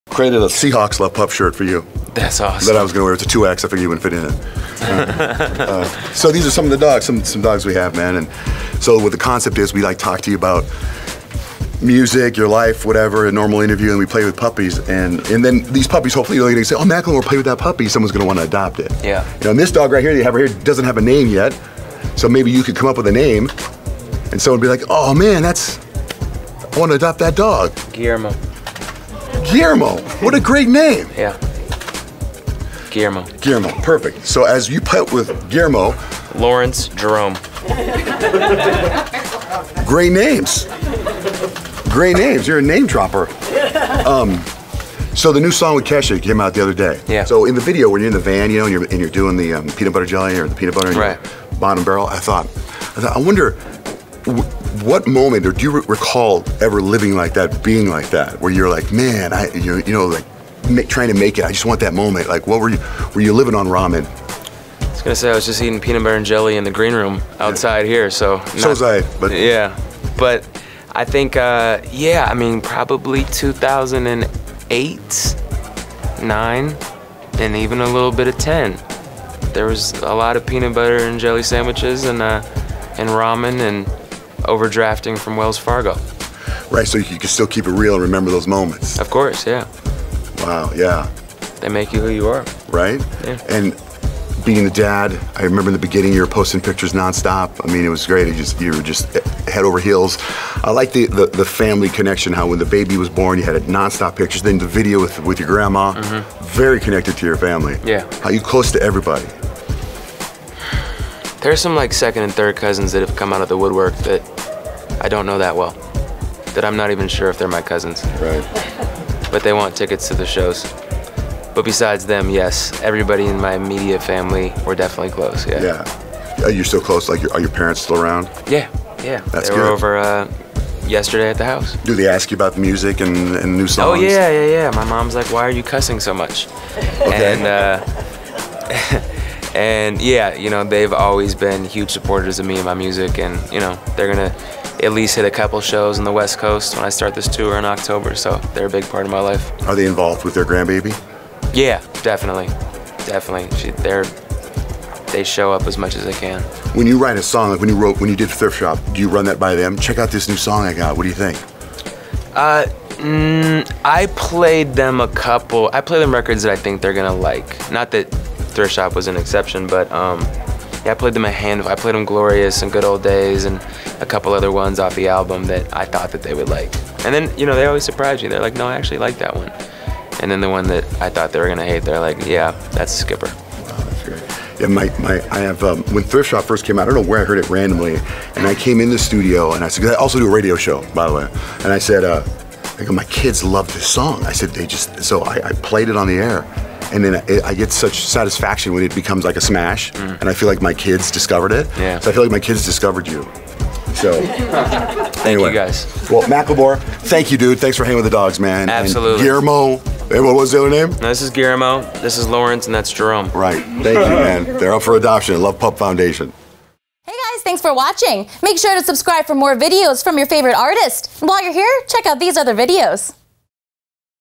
Macklemore on Fake Family, Struggle, and Puppies! | iHeartRadio Music Festival
Macklemore sits down with JohnJay and talks about how he incorporates his family into his music. He names three adoptable puppies too! Guillermo, Lawrence, and Jerome were rescued from Houston.

Watch the 2017 iHeartRadio Music Festival airing on The CW 11/22 & 11/24!